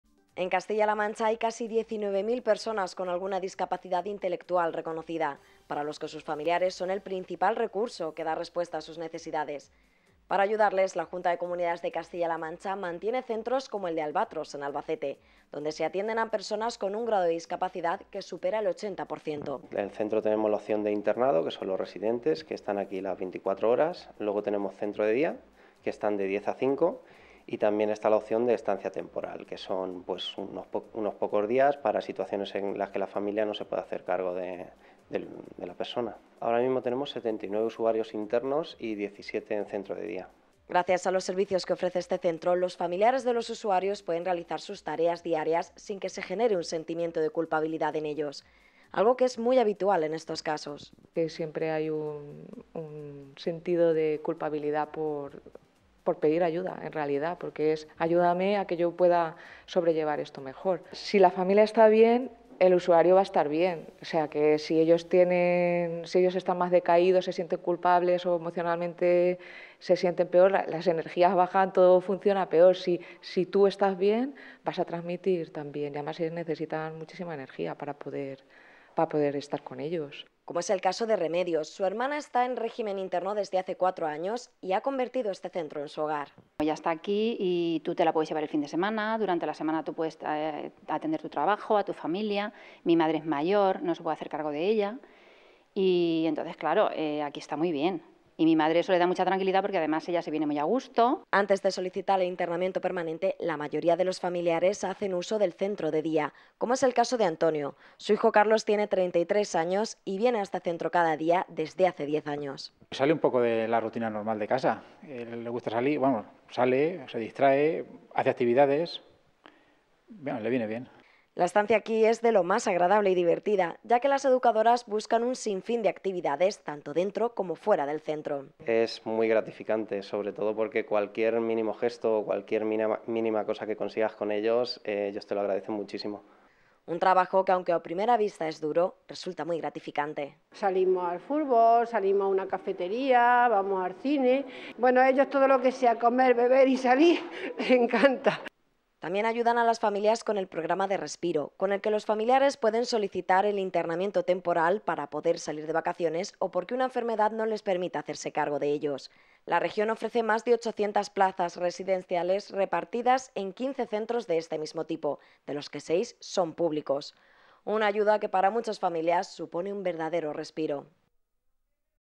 UN “RESPIRO” PARA FAMILIARES DE PERSONAS DEPENDIENTES EN ALBACETE
Casi 19.000 personas tienen alguna discapacidad intelectual en nuestra región y para ellos los familiares se convierten en imprescindibles. Con el objetivo de  aliviar la carga de la figura del cuidador cada vez son más las herramientas que se ponen sobre la mesa para usuarios y familiares. Hoy visitamos el Centro Albatros, en Albacete, que ofrece atención a 72 usuarios. Entre sus programas está el conocido como "Respiro", en el que se puede solicitar que el usuario se quede interno de forma temporal para, por ejemplo, salir de vacaciones o por enfermedad.